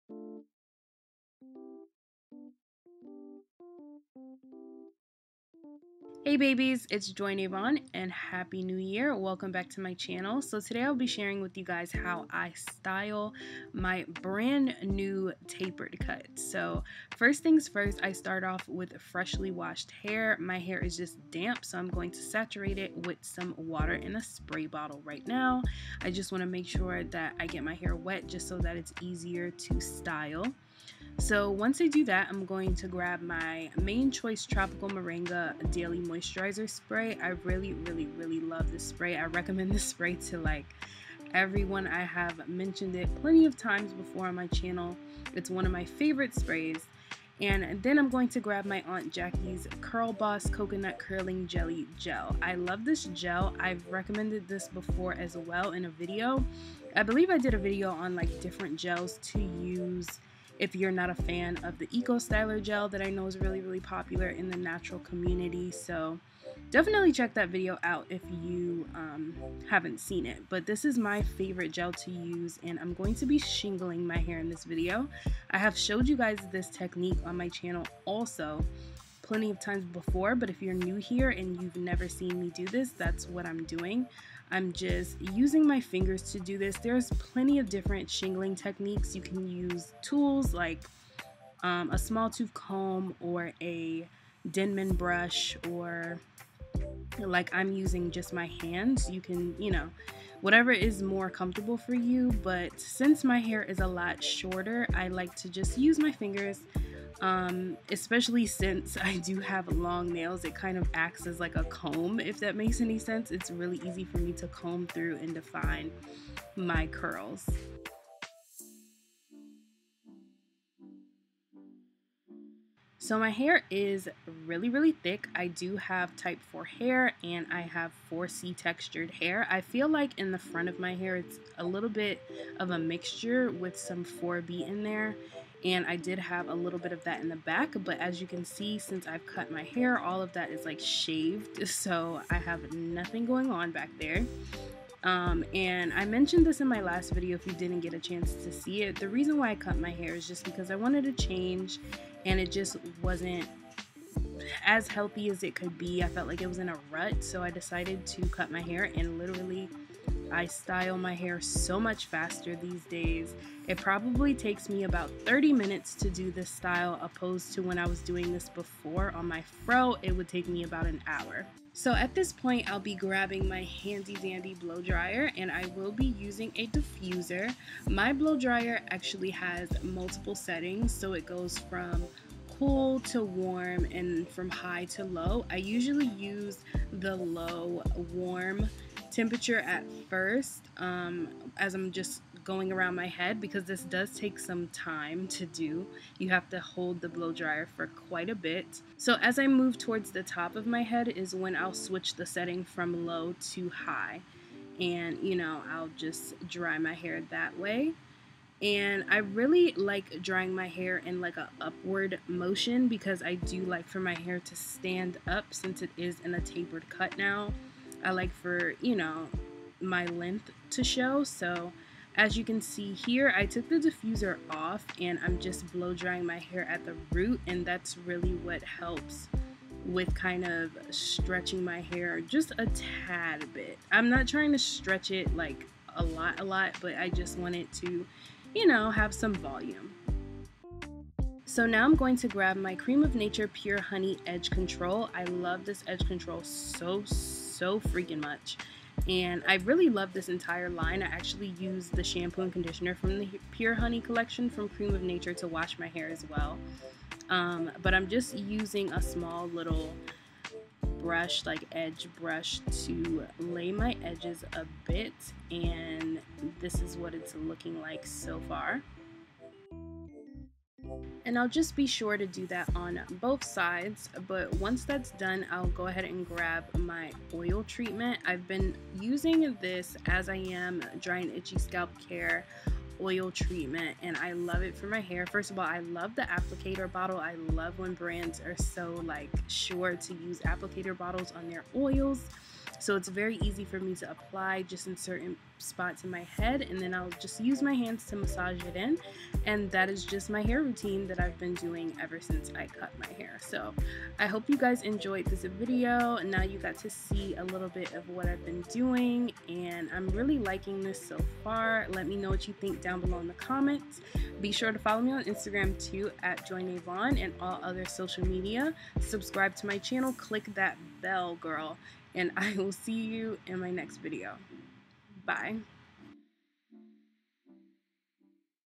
How I Style My Tapered 4C Natural Haircut! | JOYNAVON
It's my first hair tutorial of 2019!!! I'm finally sharing my current routine for my new haircut. It's simple, fast and easy.. I LOVE IT. The cut life chose me you guys, I don't know if I can turn back. What do you think?

✦ PRODUCT LISTING

The Mane Choice Daily Restorative Spray
Aunt Jackie's Coconut Curling Gelee
Creme of Nature Pure Honey Edge Control
As I Am Dry & Itchy Scalp Care Oil Treatment